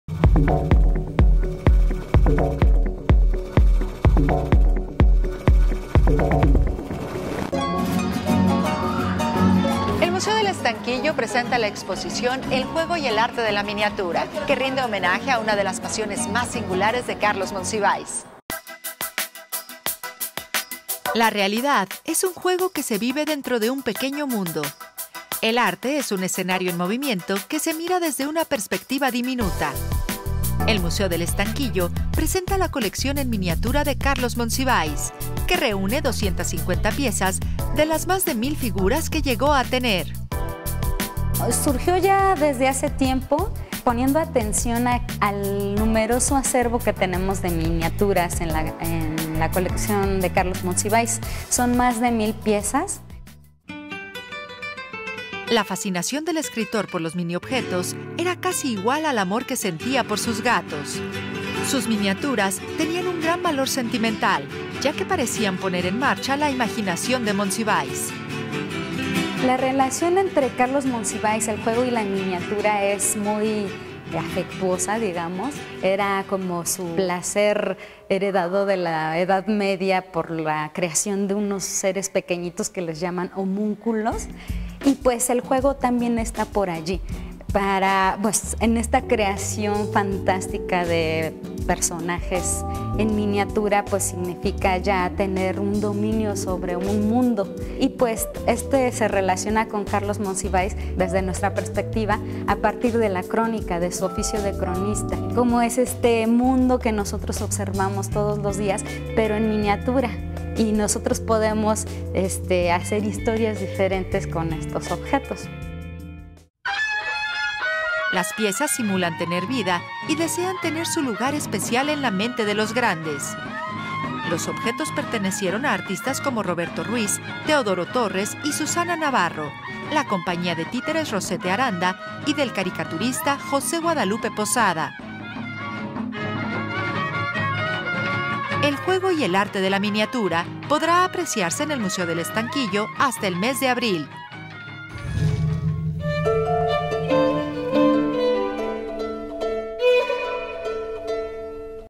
Itinerario - El juego y El arte en miniatura
“EL JUEGO Y EL ARTE EN MINIATURA”

El Museo del Estanquillo presenta la exposición “El Juego y el arte de La Miniatura“ que rinde homenaje a una de las pasiones más singulares de Carlos Monsiváis.

MUSEO DEL ESTANQUILLO
Calle Isabel la Católica #26, Centro Histórico